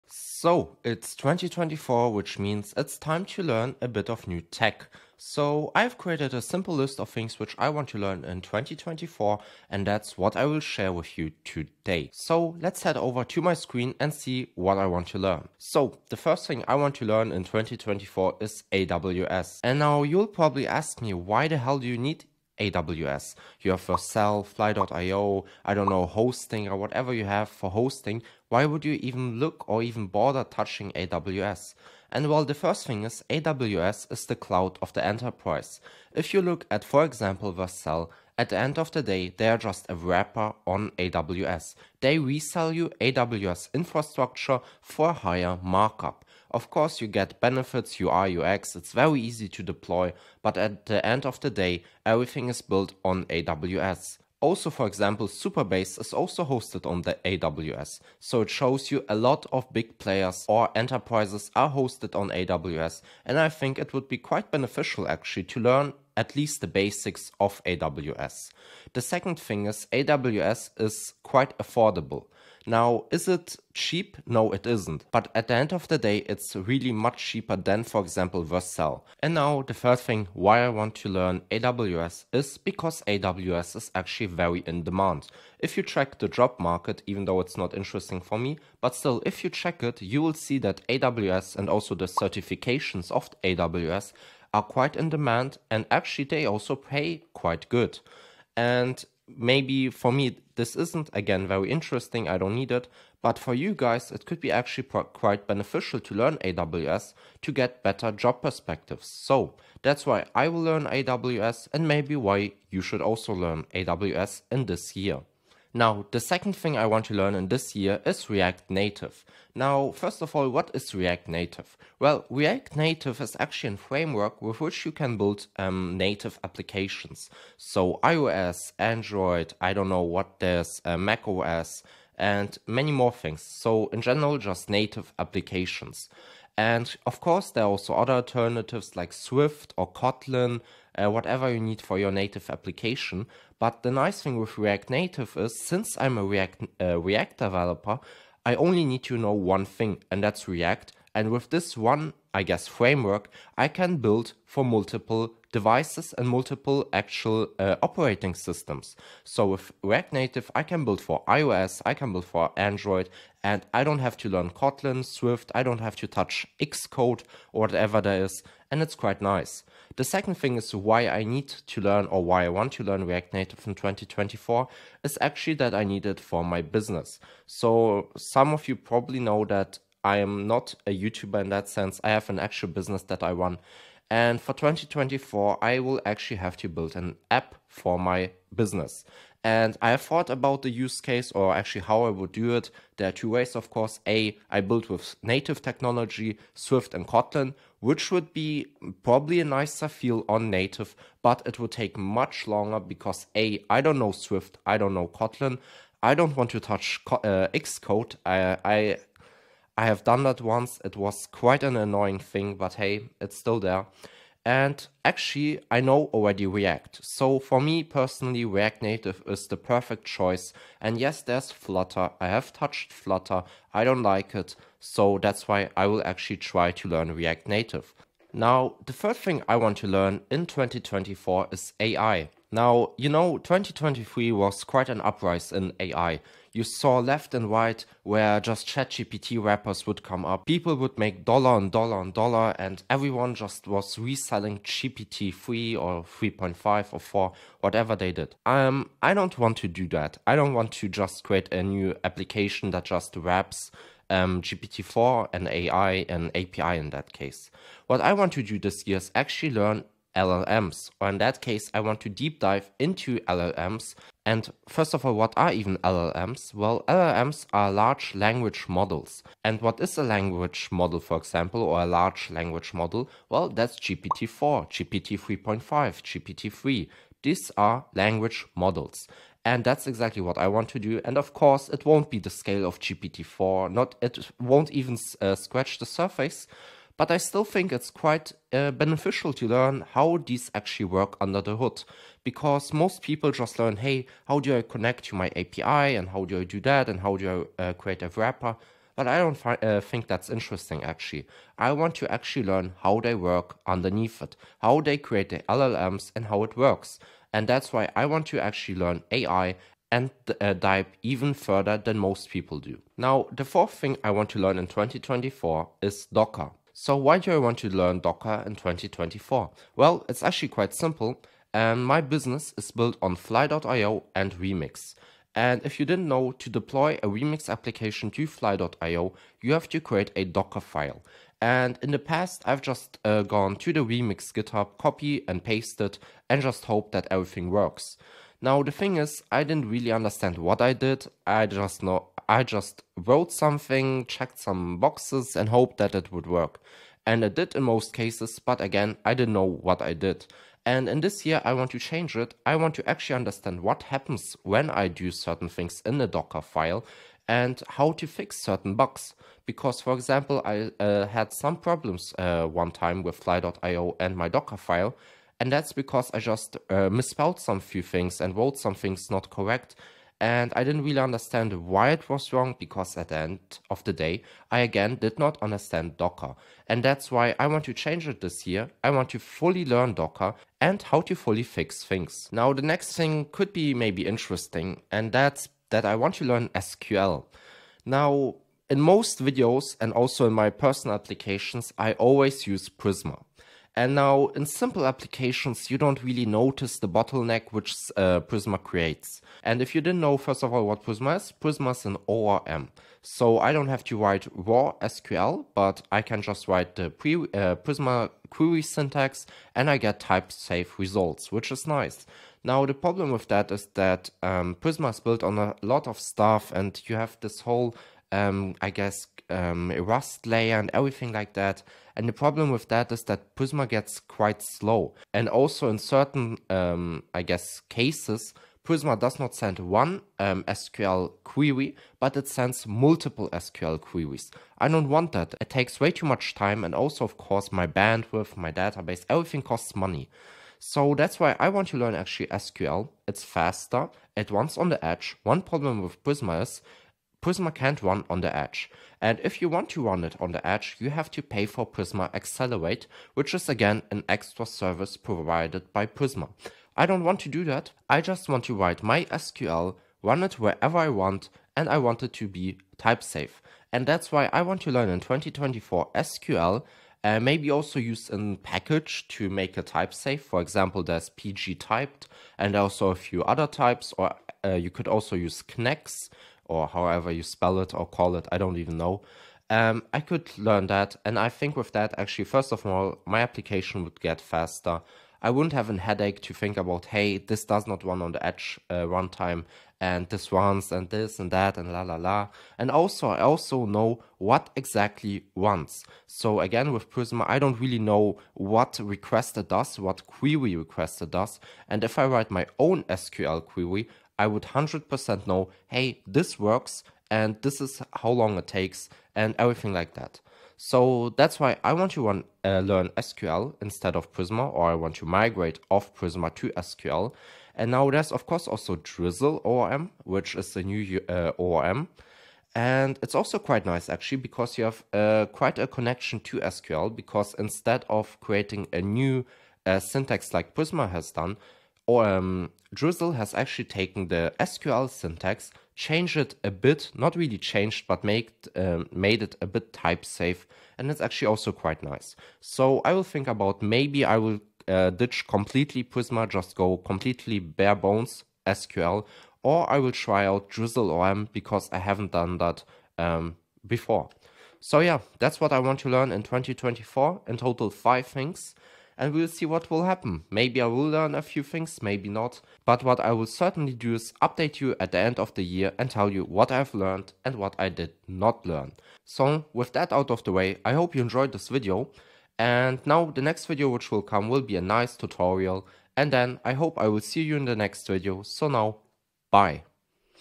Technologies I am learning in 2024
So it's 2024, which means it's time to learn new technologies. In this video, you will learn what I am planning to learn and maybe what you should also learn.